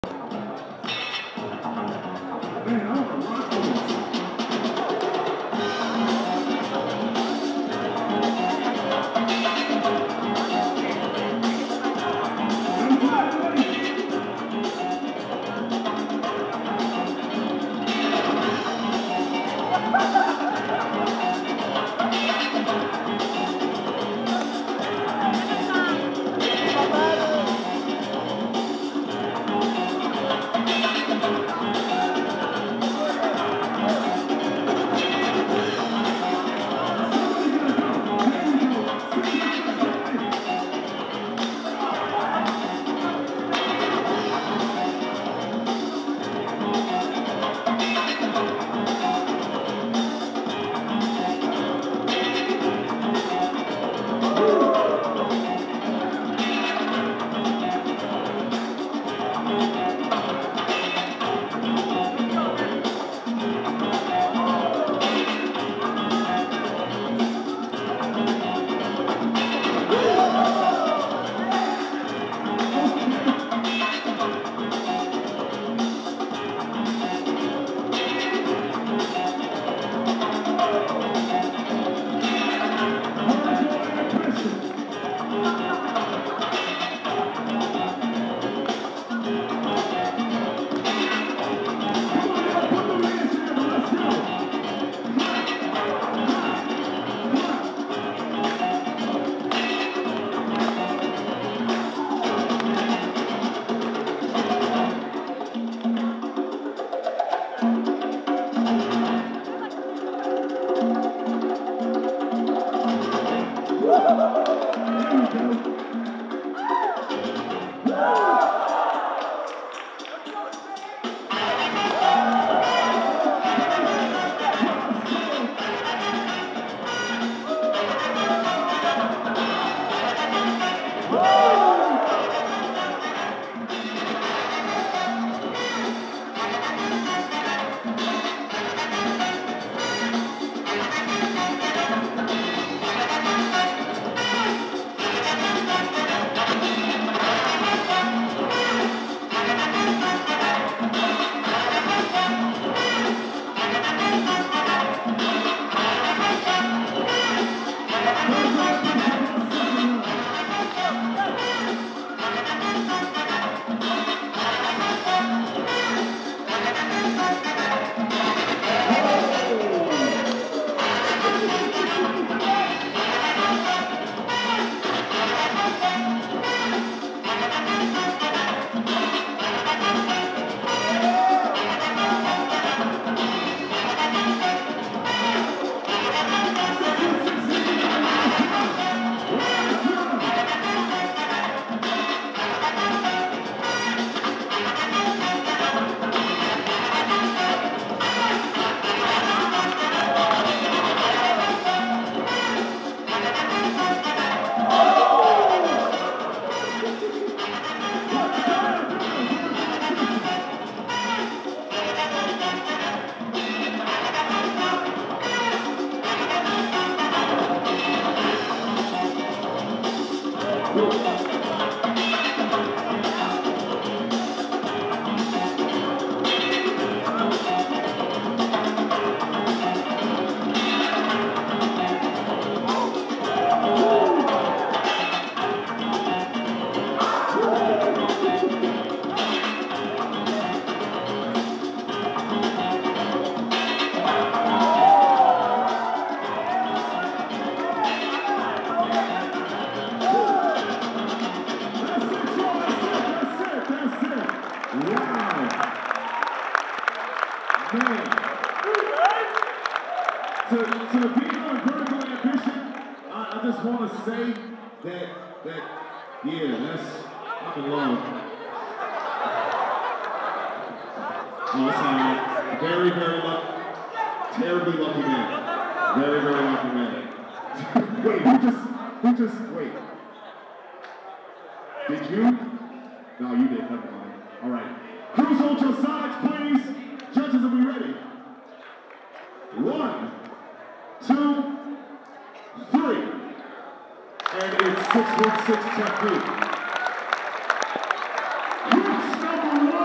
Master the Art 9 | Top 16| Hardcore 61Syx vs Vertical Ambition 2 | MIbboy.TV
MASTER THE ART 9
Hosted by Element One

Saturday - March 3rd, 2012

MC - AMS (Self-X/Boogie Knightz)

DJ - Skeme Richards (Rock Steady Crew)

Hardcore 61Syx
- Blank
- Seoul
- Nova

vs

Vertical Ambition 2
- Spuratik
- Shortee Roc

Winner: 61Syx Teknique